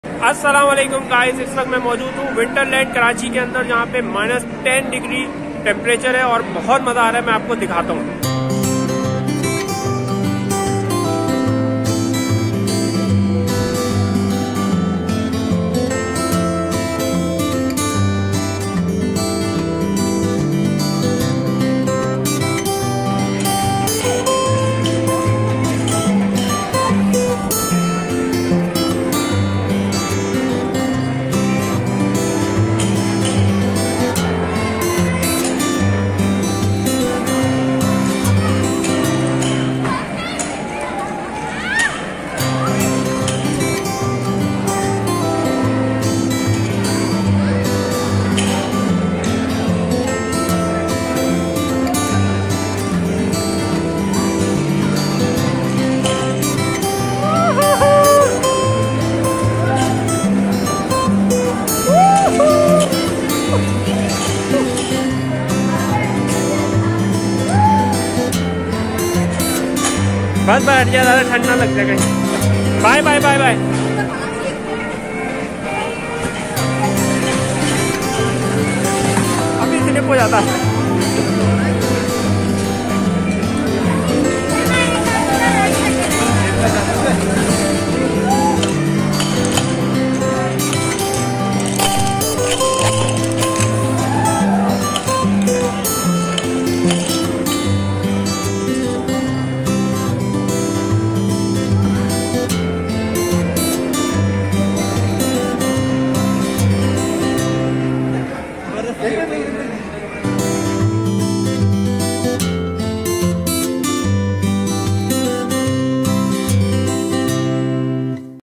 Vlog | Winterland Karachi From Inside
Join me to enjoy some wonderful rides in -10 degree temperature at winterland Karachi, Pakistan. pass is of Rs.1500 with a free meal offer as a Newyear offer. a very cool place to go with family and have fun.